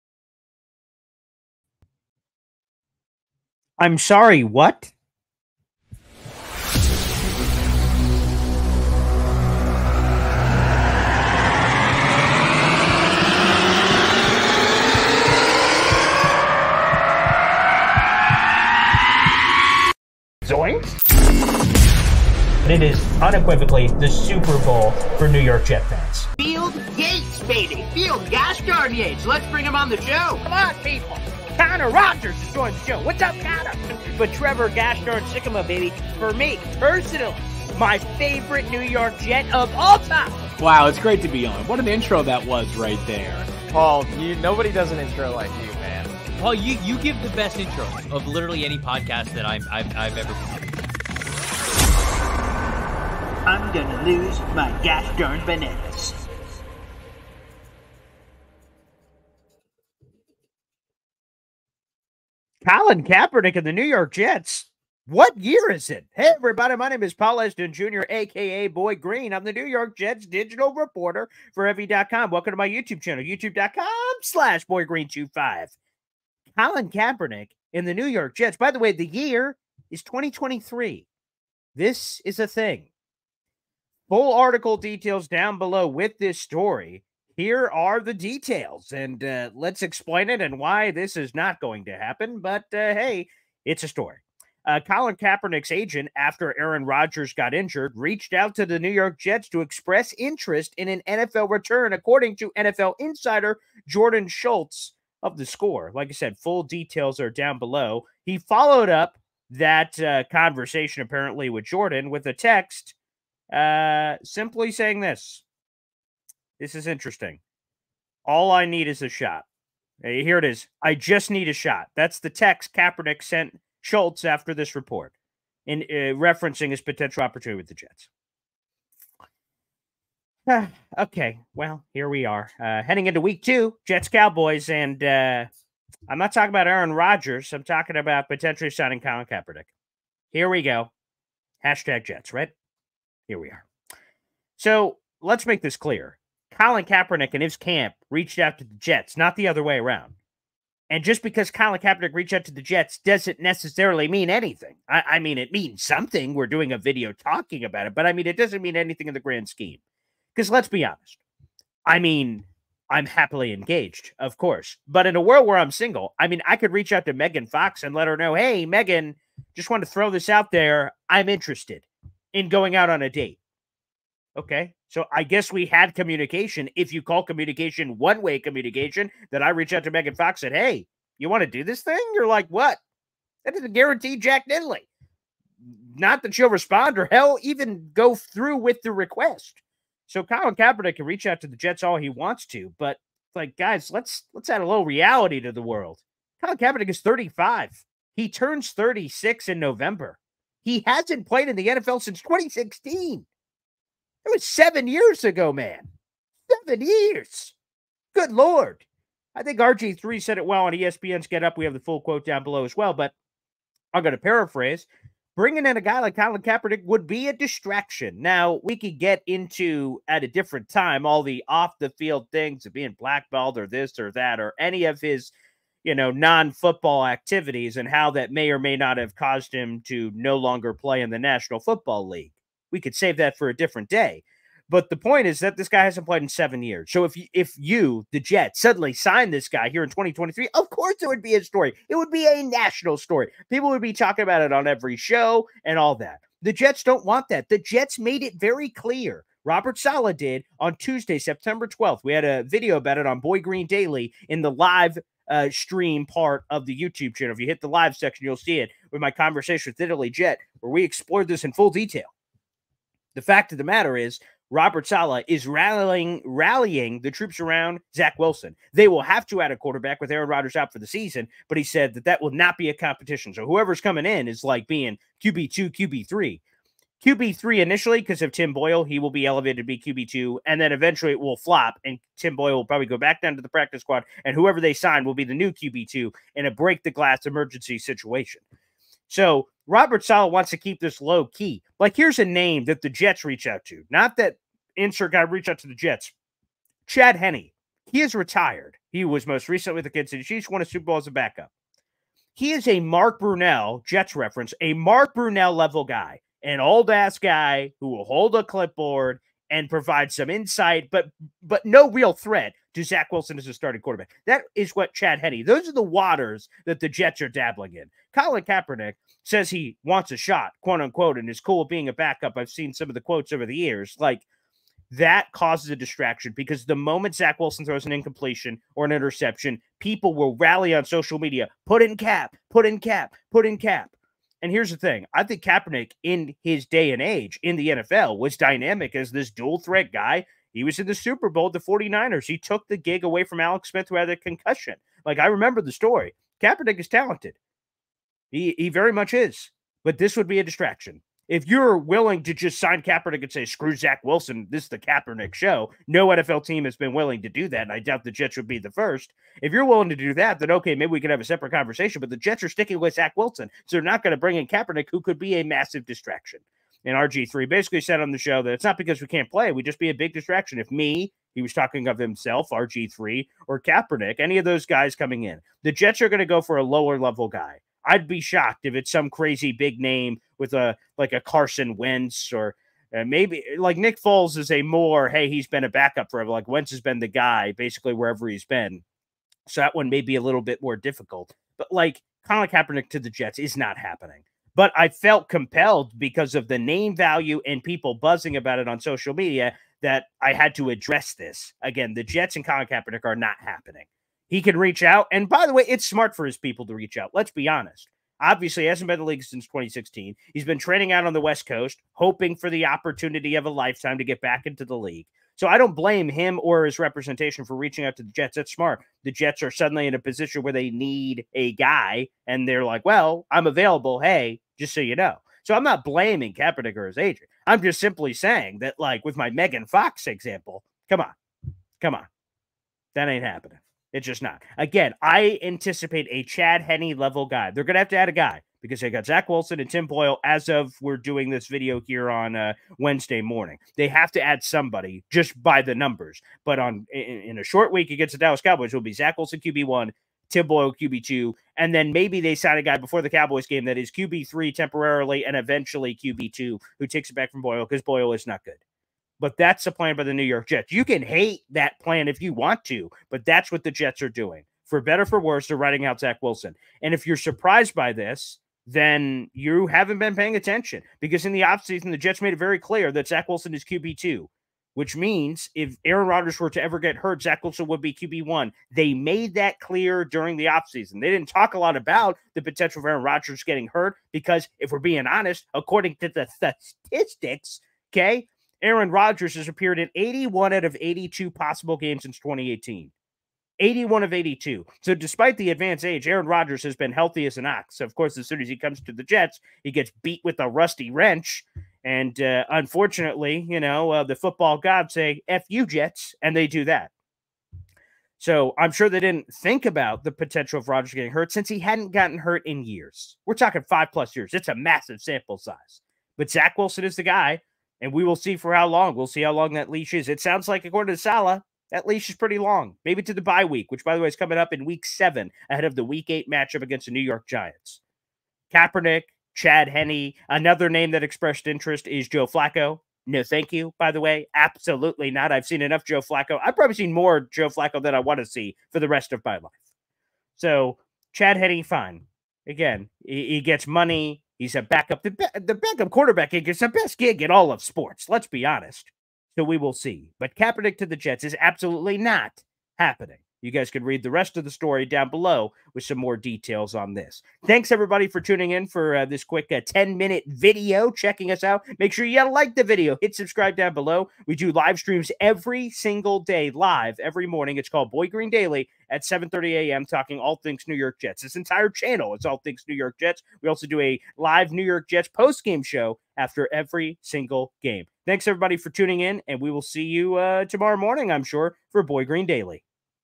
Colin Kaepernick has contacted the Jets about their QB vacancy: Report
Colin Kaepernick reached out to the Jets after they lost Aaron Rodgers for the season. Yes, this actually happened.